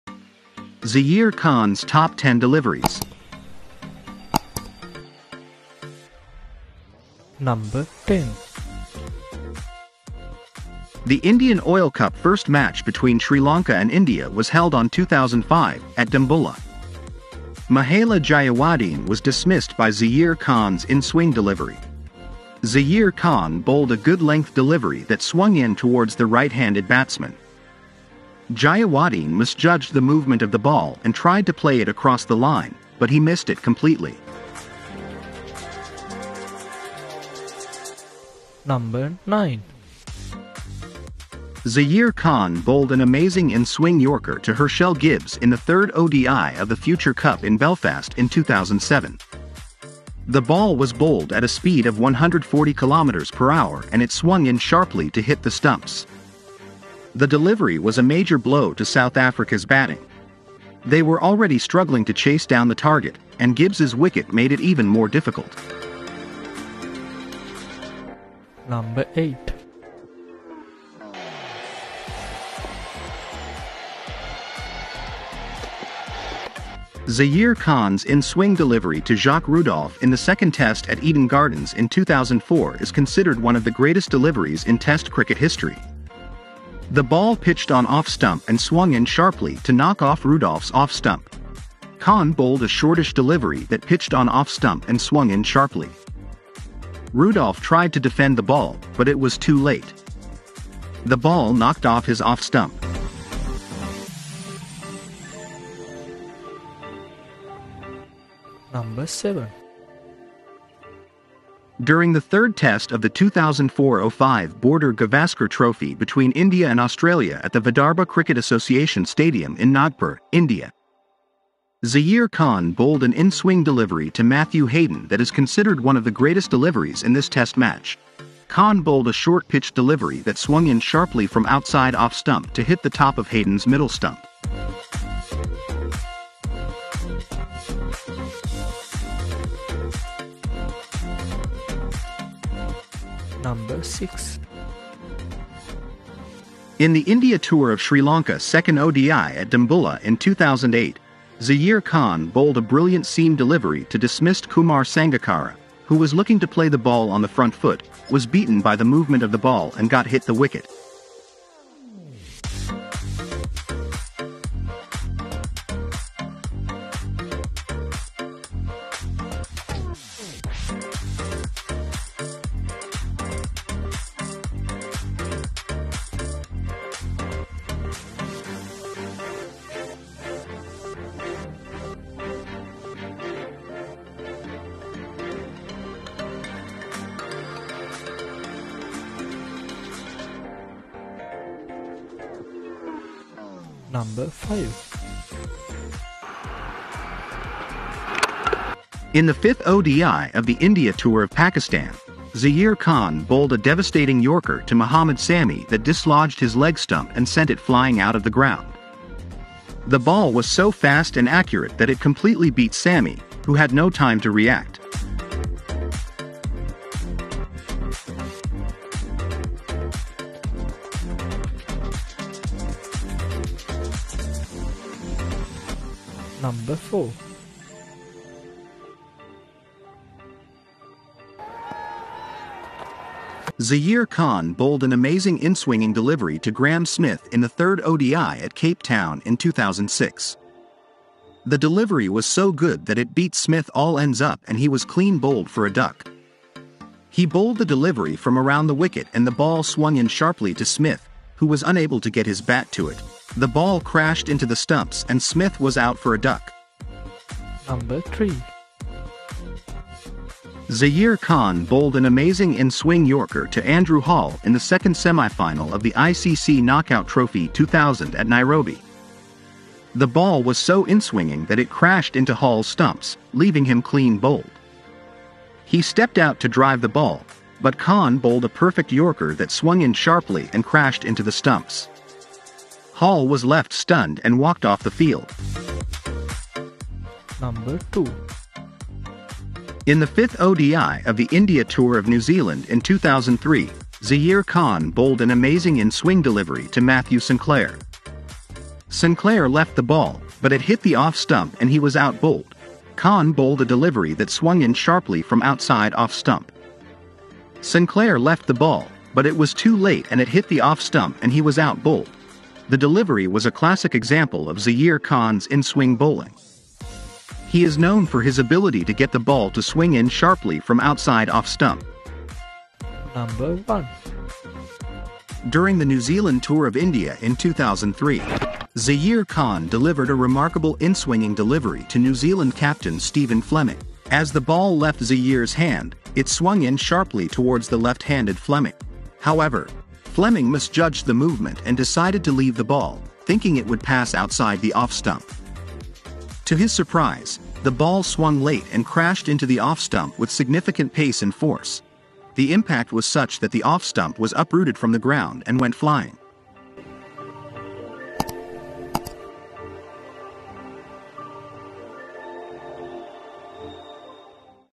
Masterclass by Zaheer Khan: Top 10 Amazing Deliveries That Left Batsmen Bewildered
Zaheer Khan TOP 10 Deliveries.

NO 10

The Indian Oil Cup 1st Match between Sri Lanka and India was held on  2005, at Dambulla. Mahela Jayawardene was dismissed by Zaheer Khan's inswing delivery.
Zaheer Khan bowled a good-length delivery that swung in towards the right-handed batsman.
Jayawardene misjudged the movement of the ball and tried to play it across the line, but he missed it completely. The ball hit the top of his middle stump, and the bails went flying, leaving Jayawardene stunned.

 NO 09

Zaheer Khan bowled an amazing inswing yorker to Herschelle Gibbs in the 3rd ODI of the Future Cup in Belfast in 2007. The ball was bowled at a speed of 140 km/h and it swung in sharply to hit the stumps.The delivery was a major blow to South Africa's batting. They were already struggling to chase down the target, and Gibbs's wicket made it even more difficult.

NO 08

Zaheer Khan's inswing delivery to Jacques Rudolph in the 2nd Test at Eden Gardens in 2004 is considered one of the greatest deliveries in Test cricket history. The ball pitched on off stump and swung in sharply to knock off Rudolph's off stump.Khan bowled a shortish delivery that pitched on off stump and swung in sharply. Rudolph tried to defend the ball, but it was too late. The ball knocked off his off stump.

NO 07

during the third Test of the 2004–05 Border-Gavaskar Trophy between India and Australia at the Vidarbha Cricket Association Stadium in Nagpur, India.Zaheer Khan bowled an inswing delivery to Matthew Hayden that is considered one of the greatest deliveries in this Test match.Khan bowled a short-pitched delivery that swung in sharply from outside off stump to hit the top of Hayden's middle stump.

NO 06

In the India tour of Sri Lanka 2nd ODI at Dambulla in 2008, Zaheer Khan bowled a brilliant seam delivery to dismissed Kumar Sangakkara.Sangakkara, who was looking to play the ball on the front foot, was beaten by the movement of the ball and got hit the wicket.

NO 05

in the fifth ODI of the India tour of Pakistan, Zaheer Khan bowled a devastating yorker to Mohammad Sami that dislodged his leg stump and sent it flying out of the ground.
The ball was so fast and accurate that it completely beat Sami, who had no time to react.

NO 04

Zaheer Khan bowled an amazing inswinging delivery to Graeme Smith in the 3rd ODI at Cape Town in 2006. The delivery was so good that it beat Smith all ends up and he was clean bowled for a duck. He bowled the delivery from around the wicket and the ball swung in sharply to Smith, who was unable to get his bat to it. The ball crashed into the stumps and Smith was out for a duck.

NO 03

Zaheer Khan bowled an amazing inswing yorker to Andrew Hall in the 2nd semi-final of the ICC Knockout Trophy 2000 at Nairobi. The ball was so inswinging that it crashed into Hall's stumps, leaving him clean bowled. He stepped out to drive the ball, but Khan bowled a perfect yorker that swung in sharply and crashed into the stumps. Hall was left stunned and walked off the field.

NO 02

In the 5th ODI of the India tour of New Zealand in 2003, Zaheer Khan bowled an amazing inswing delivery to Matthew Sinclair. Sinclair left the ball, but it hit the off stump and he was out bowled.Khan bowled a delivery that swung in sharply from outside off stump.
Sinclair left the ball, but it was too late and it hit the off stump and he was out bowled.The delivery was a classic example of Zaheer Khan's inswing bowling.
He is known for his ability to get the ball to swing in sharply from outside off stump.

NO 01

during the New Zealand tour of India in 2003. Zaheer Khan delivered a remarkable inswinging delivery to New Zealand captain Stephen Fleming.As the ball left Zaheer's hand, it swung in sharply towards the left-handed Fleming. However,
Fleming misjudged the movement and decided to leave the ball, thinking it would pass outside the off stump. To his surprise, the ball swung late and crashed into the off stump with significant pace and force.The impact was such that the off stump was uprooted from the ground and went flying.

I am really apologize for not using some Footages,